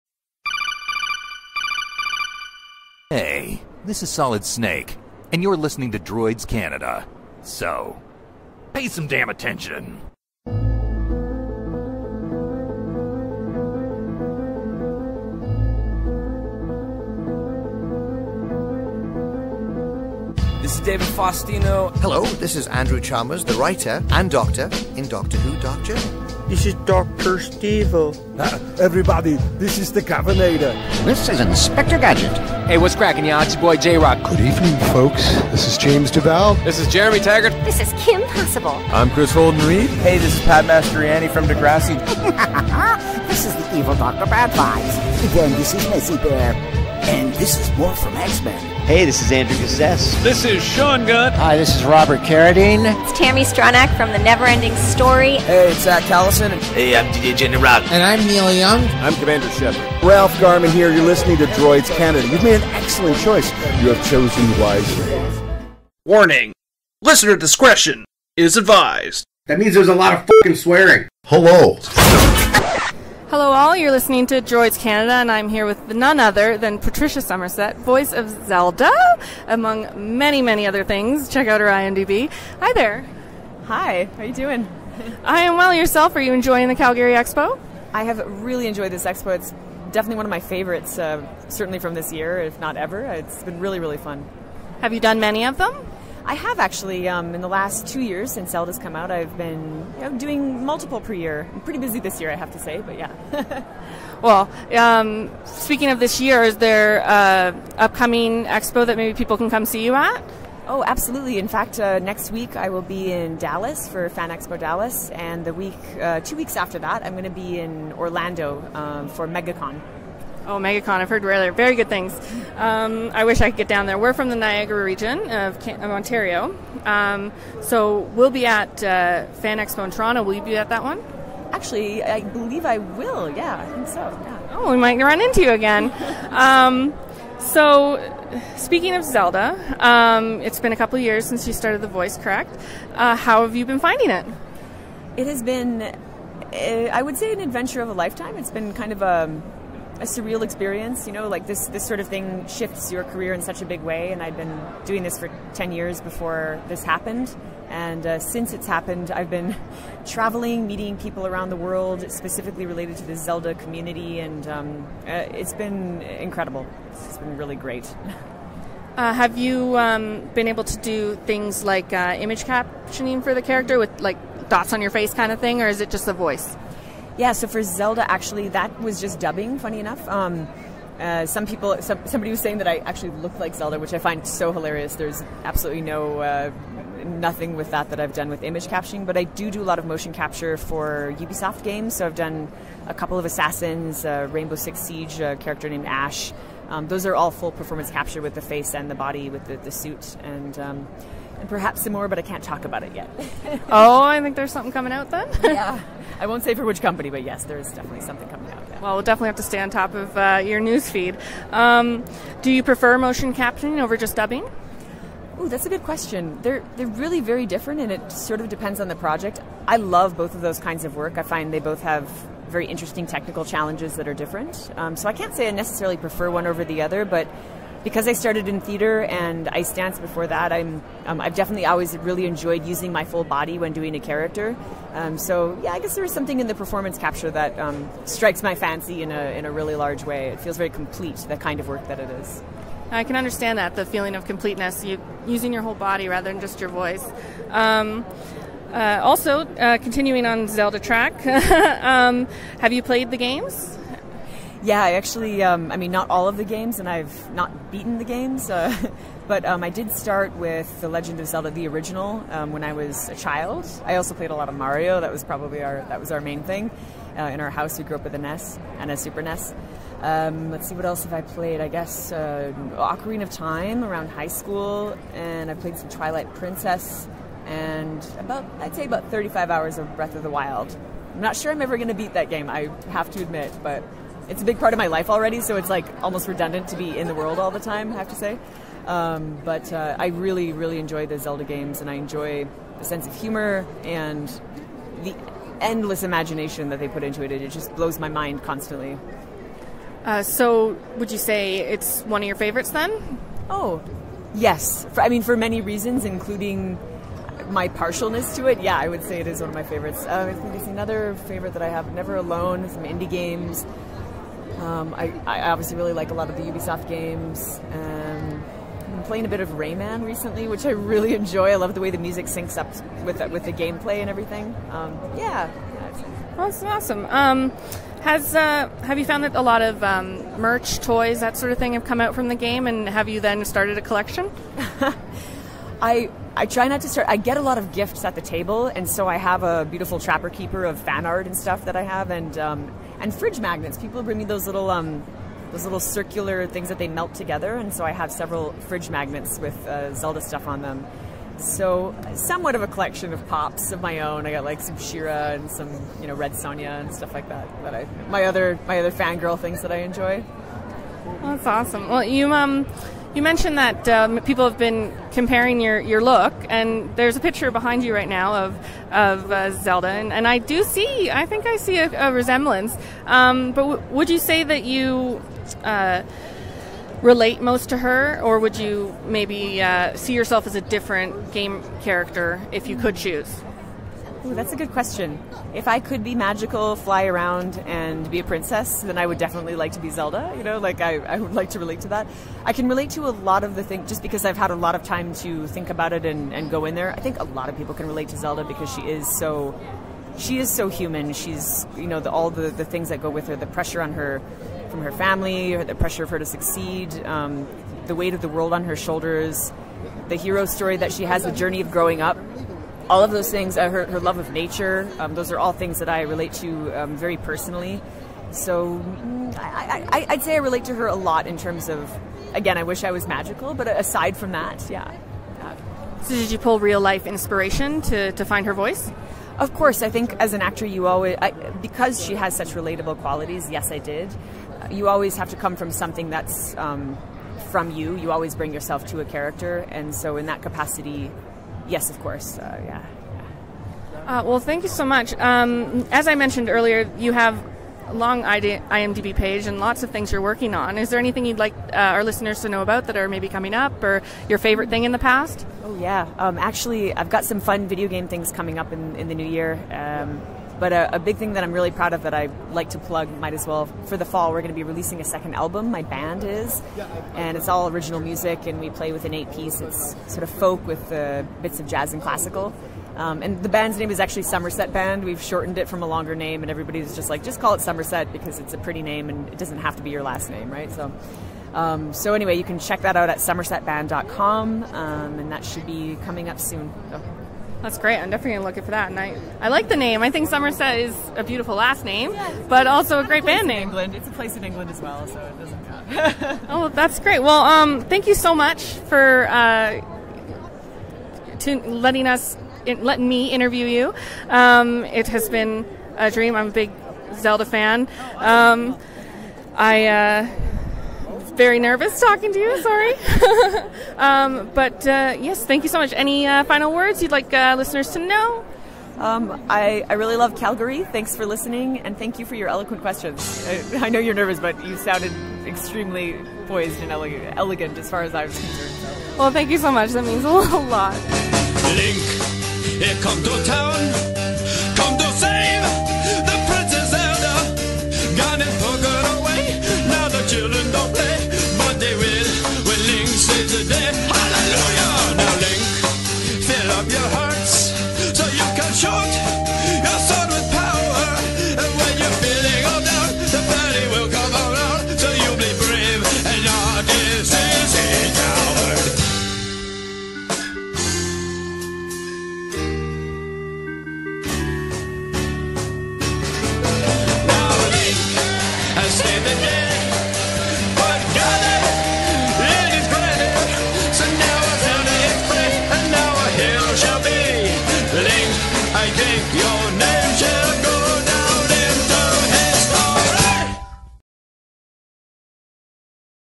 Patricia Summersett(Zelda) from The Legend of Zelda
Patricia Summersett recently became the voice of Princess Zelda in Nintendo's The Legend of Zelda: Breath of the Wild, making her the first official voice of Zelda in the game's thirty-year franchise.Patricia has also provided full performance capture for two assassins in the Assassin’s Creed Franchise (Hope Jensen &amp; Galina Voronina) as well as for 'Ash' in Rainbow Six: Siege. She has also appeared in films such as Mother! and The Saver and was the onset puppeteer/voice of Smurfette, Vexy and Clumsy during the making of the Smurfs II movie.